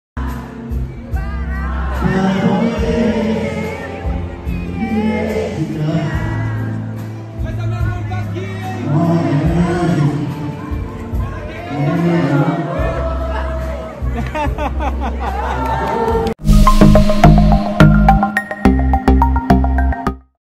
Roberto Carlos manda fã calar a boca durante show no Rio de Janeiro
Roberto Carlos se irritou com um fã durante um show no Rio de Janeiro e o mandou calar a boca. O fato foi flagrado em vídeo na noite dessa quarta-feira (13). Ele se irritou com fãs que foram à beira do palco antes do "tempo previsto".